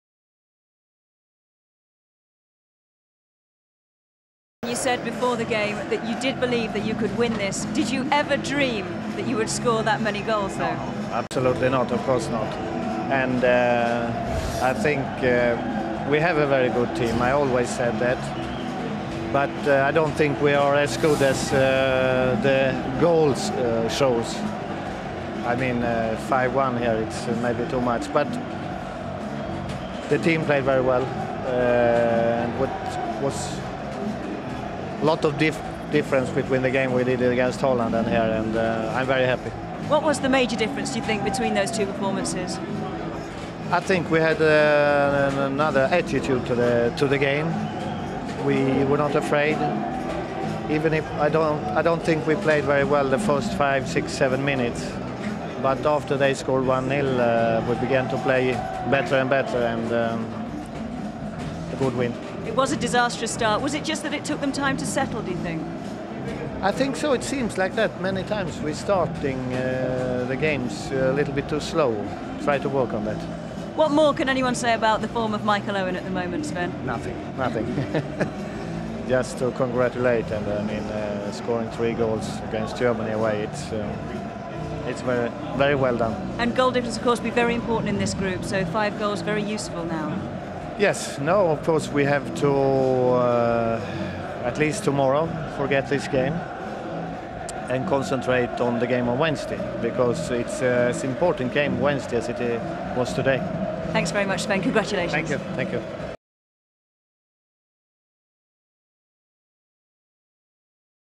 Germany 1-5 England - Sven-Goran Eriksson's reaction
A hat-trick from Michael Owen and goals from Steven Gerrard & Emile Heskey gave England one of their greatest performances as they beat Germany 5-1 in Munich.